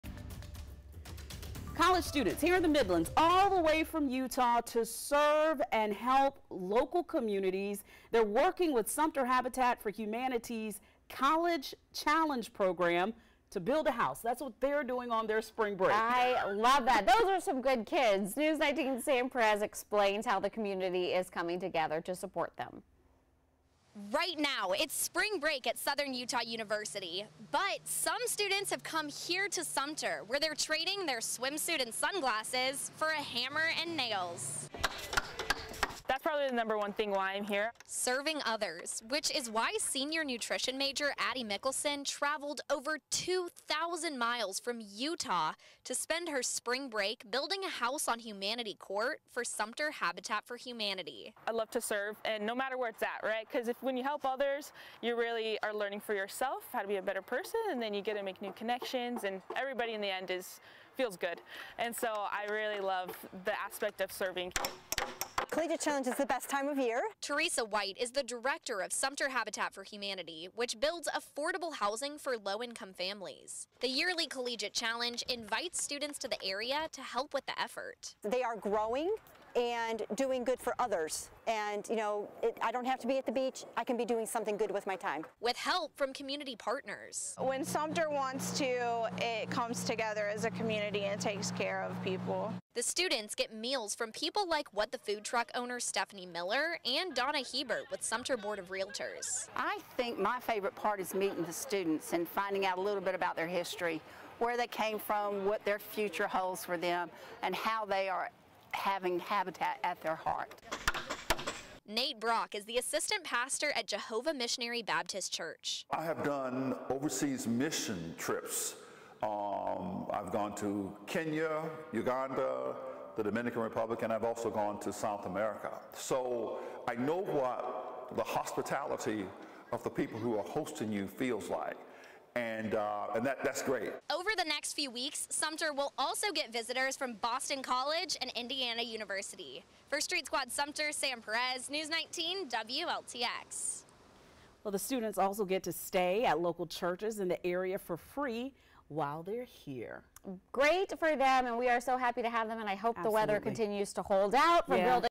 College students from near and far helping the community
College students from the Midlands and as far away as Utah are helping Habitat for Humanity in Sumter.